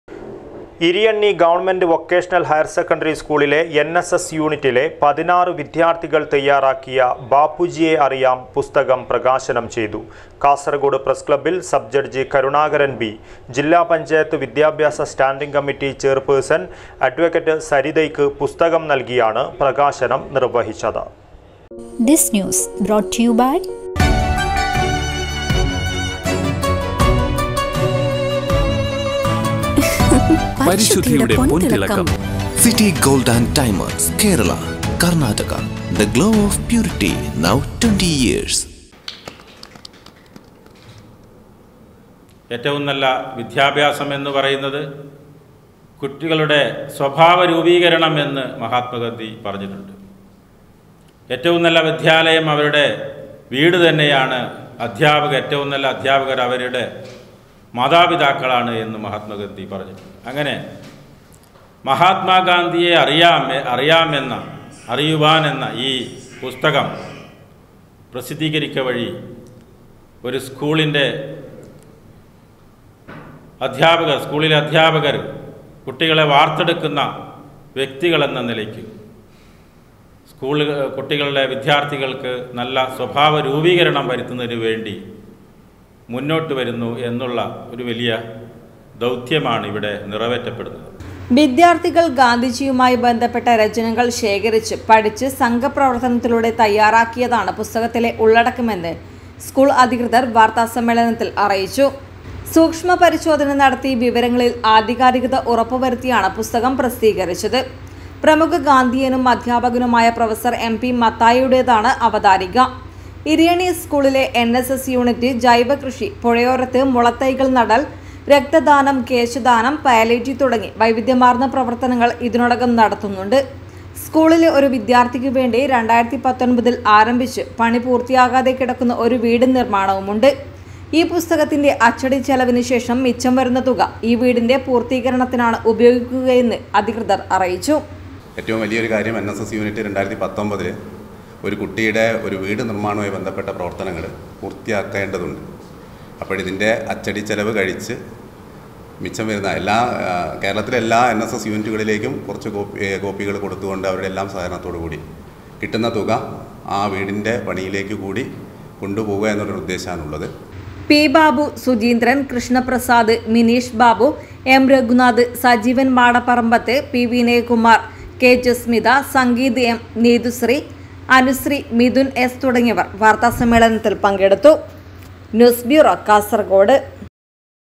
ഇരിയണ്ണി സ്‌കൂളിലെ വിദ്യാര്‍ഥികള്‍ തയ്യാറാക്കിയ 'ബാപ്പുജിയെ അറിയാം' പുസ്തകം പ്രകാശനം ചെയ്തു
ഇരിയണ്ണി സ്‌കൂളിലെ എന്‍ എസ് എസ്  യൂനിറ്റിലെ 16 വിദ്യാര്‍ഥികള്‍ തയ്യാറാക്കിയ 'ബാപ്പുജിയെ അറിയാം' പുസ്തകം പ്രകാശനം ചെയ്തു. കാസര്‍കോടി പ്രസ് ക്ലബ്ബിലായിരുന്നു പ്രകാശന ചടങ്ങ്.